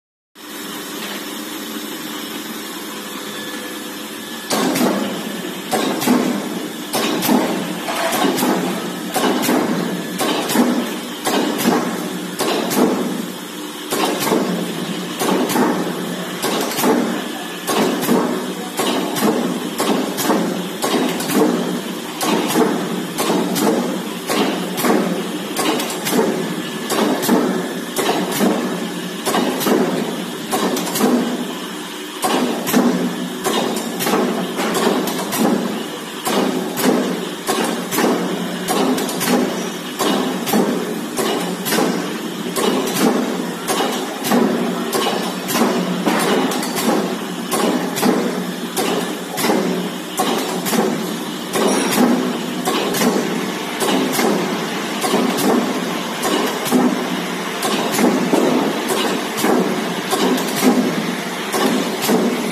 1260 KOMATSU JAPAN MODEL: OBS-80-3B C’ FRAME SINGLE CRANK PRESS/3
FOR SALE :
USED/GOOD CONDITION
KOMATSU JAPAN MODEL: OBS-80-3B
‘C’ FRAME SINGLE CRANK PRESS

Brief Specifications/ Details
SKU # 1260
Cap: 80Tons
Stroke: 130mm
Die Height: 350mm
Ram Size: 550 x 450mm
Bed Size: 1000 x 600mm